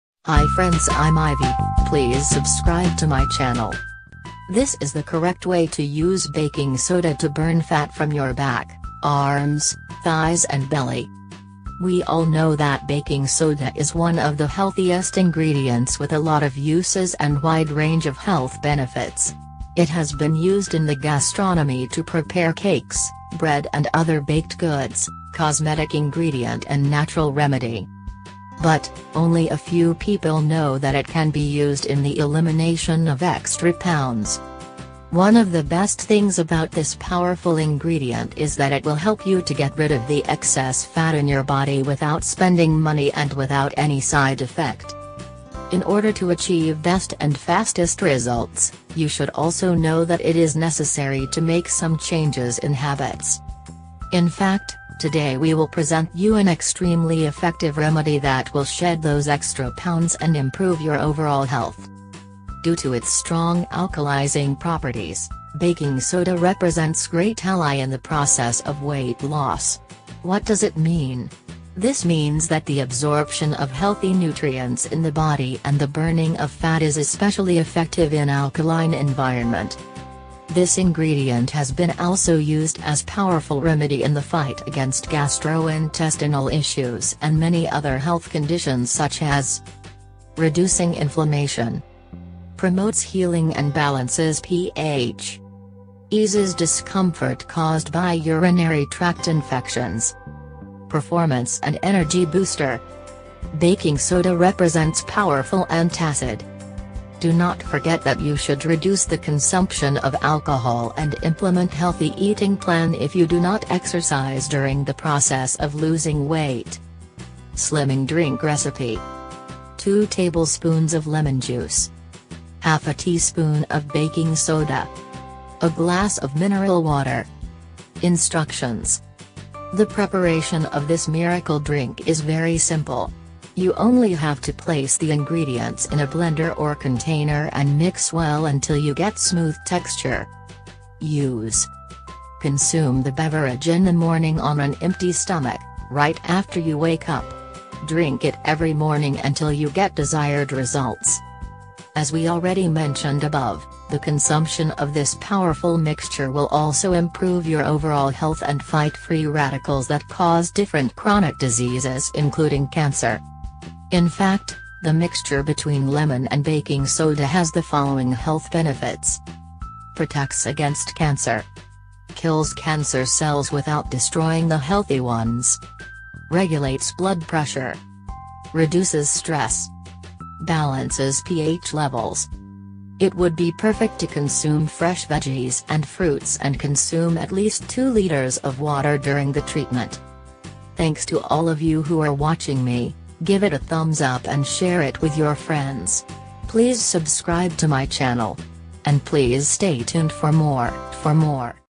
This Is The Correct Way To Use # Baking Soda,,, To Burn Fat From Your Back, Arms, Thighs And Belly
➤We all know that baking soda is one of the healthiest ingredients with many uses and many health benefits. It has been used in gastronomy to prepare cakes, bread and other baked goods, cosmetic ingredients and natural remedies!

But, only some people know that it can be used to remove extra pounds.

One of the best things about this potent ingredient is that it will help you get rid of excess fat in your body without spending money and without any side effects.

To get the best results and the fastest, you should also know that you need to make some changes in the habits.

In fact, today we are going to present you an extremely effective remedy that will help you lose those extra pounds and improve your overall health.

Due to its strong alkalizing properties, baking soda is a great ally in the weight loss process! What does it mean? This means that absorption of healthy nutrients in the body and fat burning is particularly effective in an alkaline environment.

This ingredient has also been used as a powerful remedy in the fight against gastrointestinal problems and many other health conditions such as:

Reduce inflammation
Promotes healing and balances pH
Relieves discomfort caused by urinary tract infections
Performance and Power Enhancer
Baking soda is a powerful antacid
Do not forget that you should reduce alcohol consumption and implement a healthy diet plan if you do not exercise during the weight loss process.

Slimming Drink Recipe

2 tablespoons lemon juice
½ teaspoon baking soda
A glass of mineral water

instructions:

The preparation of this miracle drink is very simple. Just place the ingredients in a blender or bowl and mix well until you get a soft texture!

use:

Drink the drink in the morning on an empty stomach just after waking up. Take it every morning until you get the desired results.

As we have already mentioned, the consumption of this powerful blend will also improve your overall health and fight the free radicals that cause various chronic diseases, including cancer.

In fact, the mixture between lemon and baking soda has the following health benefits:

Protects against cancer
Kill cancer cells without destroying healthy cells
Regulates blood pressure
Reduces stress
Balances pH levels
It would be PERFECT to consume fresh vegetables and fruits and consume at least two liters of water during treatment!

♦ I hope you enjoy this video. And I think the information will be useful for you all.

♥ Ú all and Wish you best of Luck.
(Nature for Health-natureforhealth)